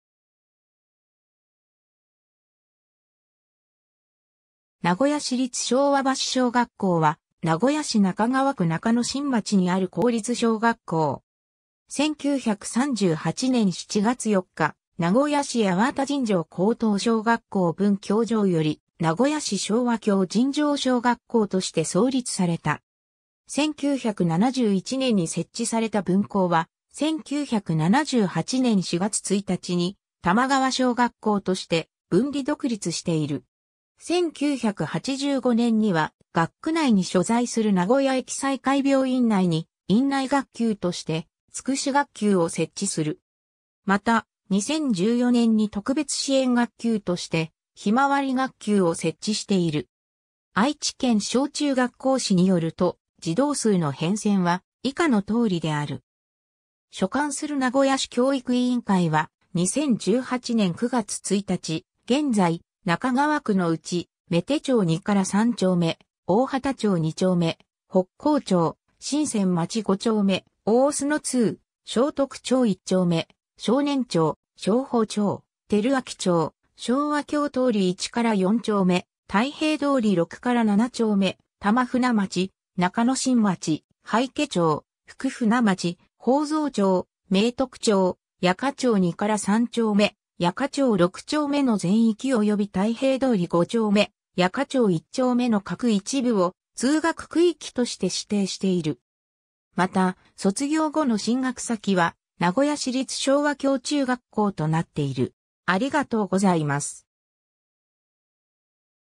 名古屋市立昭和橋小学校
名古屋市立昭和橋小学校（なごやしりつ しょうわばししょうがっこう）は、名古屋市中川区中野新町にある公立小学校。
 1938年（昭和13年）7月4日、名古屋市八幡尋常高等小学校分教場より名古屋市昭和橋尋常小学校として創立された。
 1971年（昭和46年）に設置された分校は、1978年（昭和53年）4月1日に玉川小学校として分離独立している。
 1985年（昭和60年）には、学区内に所在する名古屋掖済会病院内に院内学級として「つくし学級」を設置する。
また、2014年（平成26年）に特別支援学級として「ひまわり学級」を設置している。
 『愛知県小中学校誌』（2018年）によると、児童数の変遷は以下の通りである。
 所管する名古屋市教育委員会は、2018年（平成30年）9月1日現在、中川区のうち、馬手町2～3丁目・大畑町2丁目・北江町・清船町5丁目・小碓通・正徳町1丁目・松年町・正保町・昭明町・昭和橋通1～4丁目・太平通6～7丁目・玉船町・中野新町・葉池町・福船町・法蔵町・明徳町・八家町2～3丁目・八家町6丁目の全域および太平通5丁目・八家町1丁目の各一部を通学区域として指定している。
 また、卒業後の進学先は名古屋市立昭和橋中学校となっている。